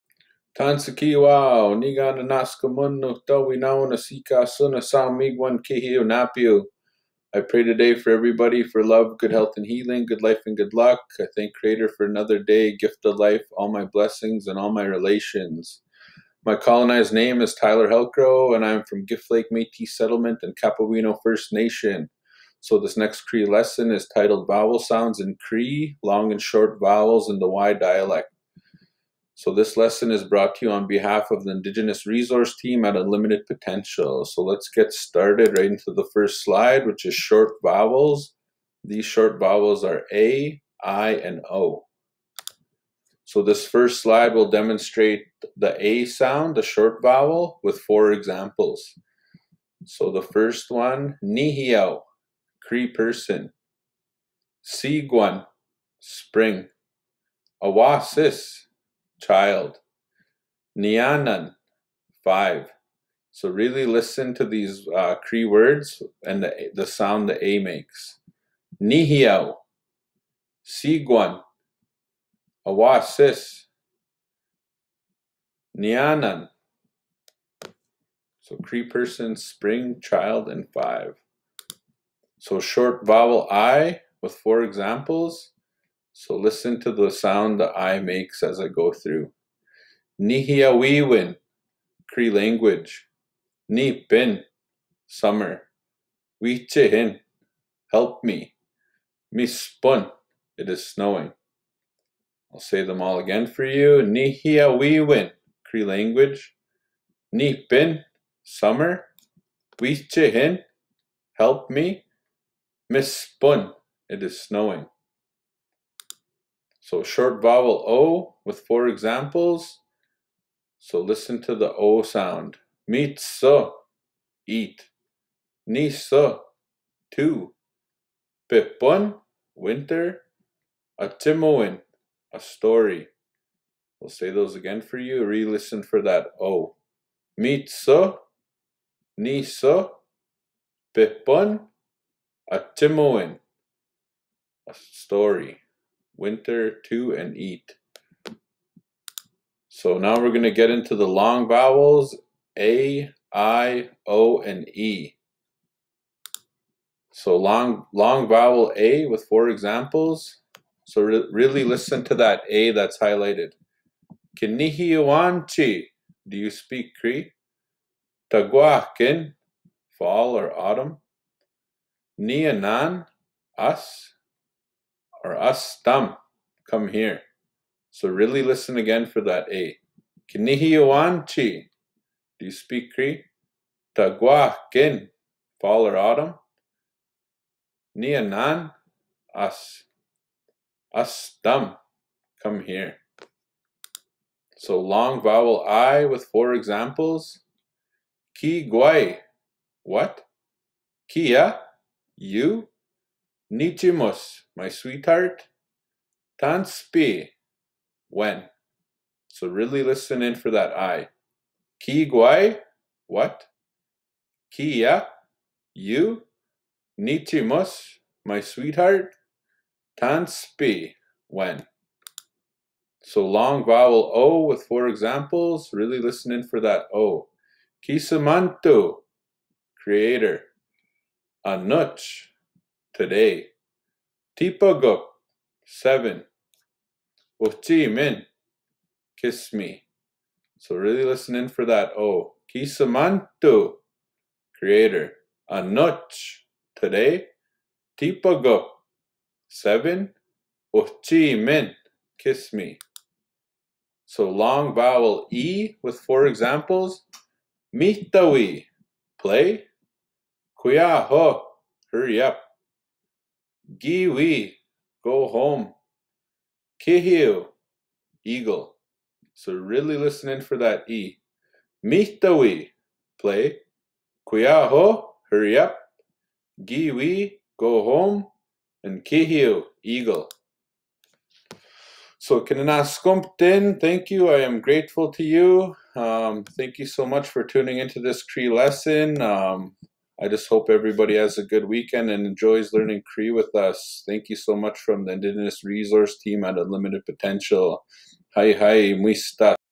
Cree language learning-Vowel Sounds in Cree
Cree language vowel sounds-long and short vowels y-dialect